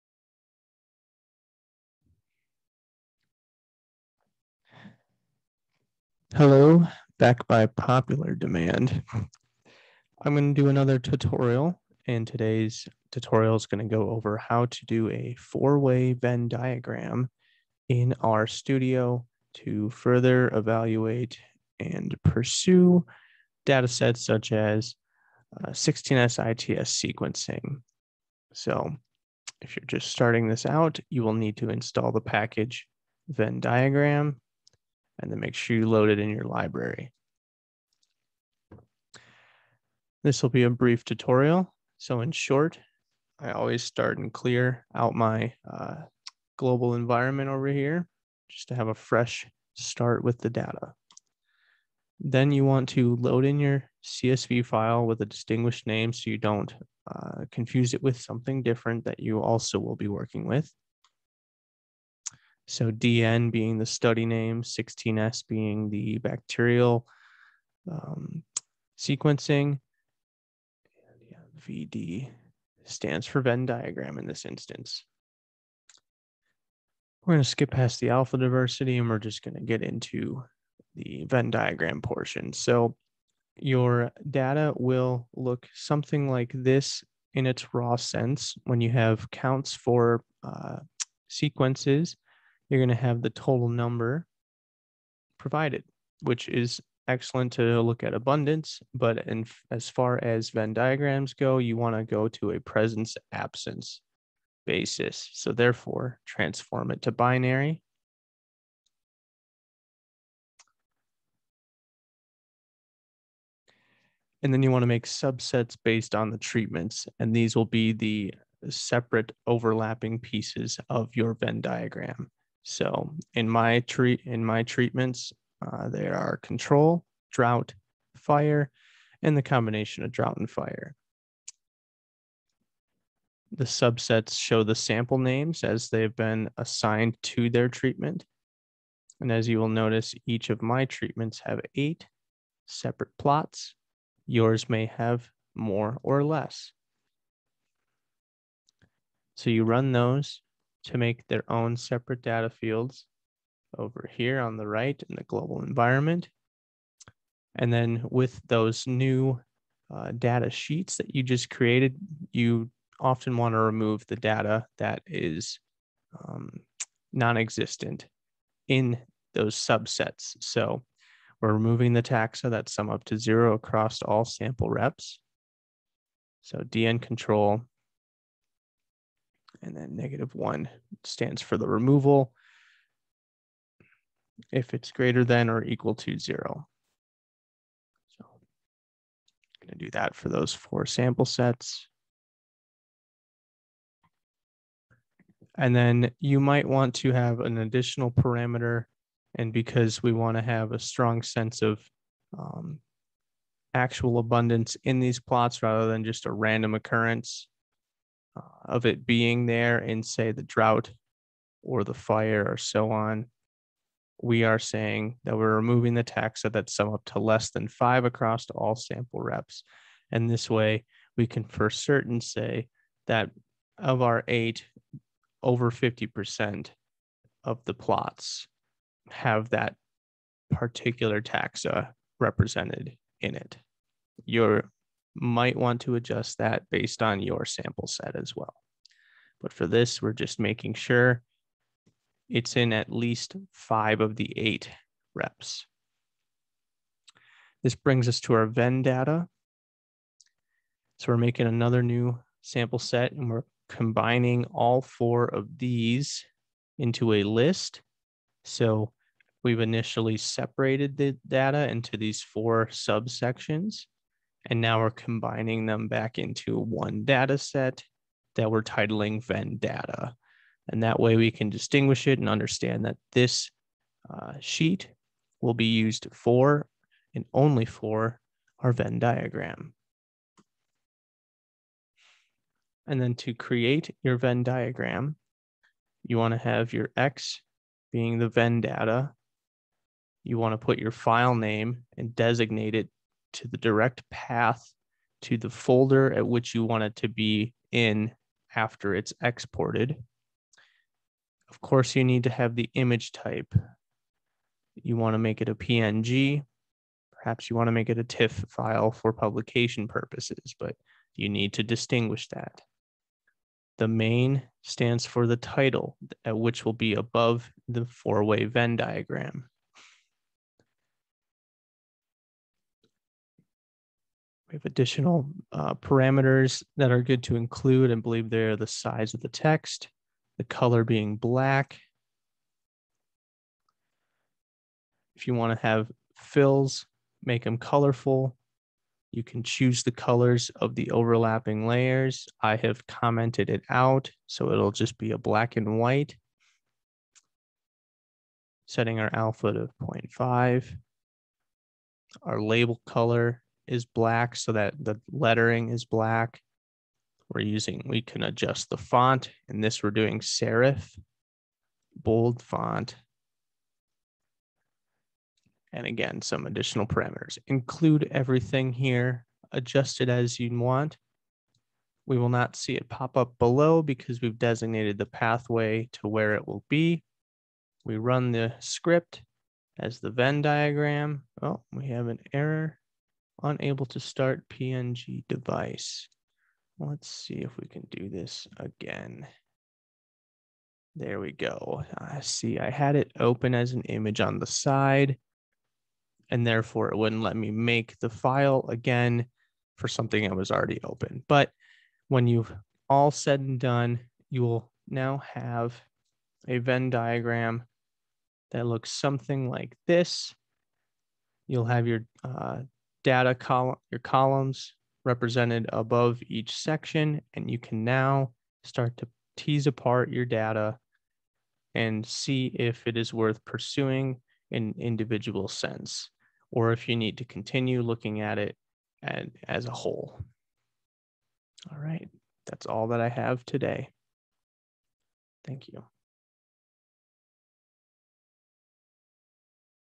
Rstudio 4-way Venn Diagram Tutorial
In this video I walk you through how to make a 4-way Venn Diagram with your data.

I am not a biostatistician, but I have my B.A. in Biology and my M.S. in Soil Science. Much of my work in R has been self-taught (with major components given to me to work off of) and I just hope I can offer some guidance through simple tasks in R that often can result in major headaches.

Please ask questions if you have them!